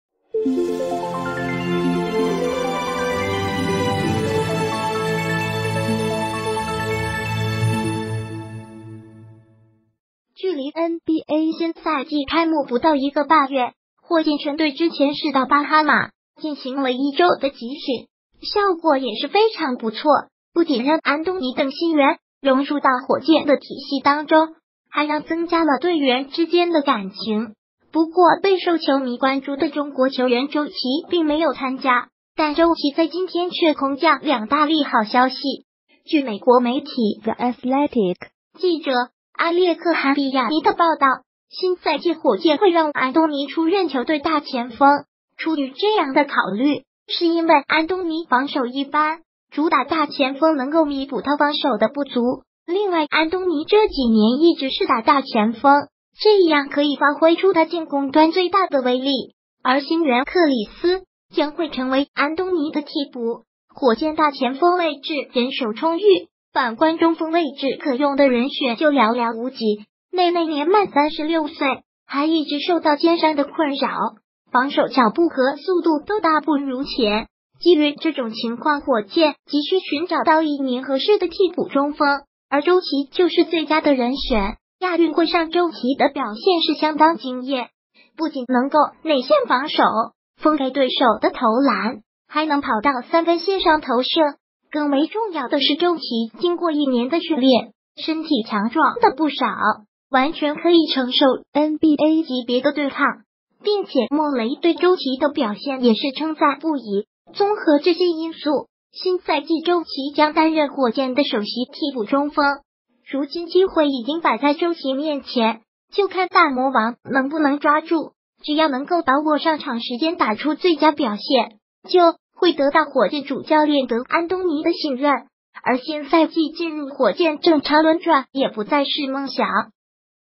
周琦空降两大利好消息，大魔王又迎上位良机，将成火箭新一代饼皇
距离NBA新赛季开幕不到一个半月，火箭全队之前是到巴哈马进行了一周的集训，效果也是非常不错，不仅让安东尼等新援融入到火箭的体系当中，还让增加了队员之间的感情，不过备受球迷关注的中国球员周琦并没有参加，但周琦在今天却空降两大利好消息。据美国媒体《The Athletic》记者阿列克汗·比亚尼的报道，新赛季火箭会让安东尼出任球队大前锋，出于这样的考虑是因为安东尼防守一般，主打大前锋能够弥补他防守的不足，另外安东尼这几年一直是打大前锋，这样可以发挥出他进攻端最大的威力，而新援克里斯将会成为安东尼的替补。火箭大前锋位置人手充裕，反观中锋位置可用的人选就寥寥无几，内内年满36岁，还一直受到肩伤的困扰，防守脚步和速度都大不如前，基于这种情况火箭急需寻找到一名合适的替补中锋，而周琦就是最佳的人选。亚运会上周琦的表现是相当惊艳，不仅能够内线防守，封盖对手的投篮，还能跑到三分线上投射，更为重要的是周琦经过一年的训练，身体强壮的不少，完全可以承受NBA级别的对抗，并且莫雷对周琦的表现也是称赞不已，综合这些因素，新赛季周琦将担任火箭的首席替补中锋。如今机会已经摆在周琦面前，就看大魔王能不能抓住，只要能够把握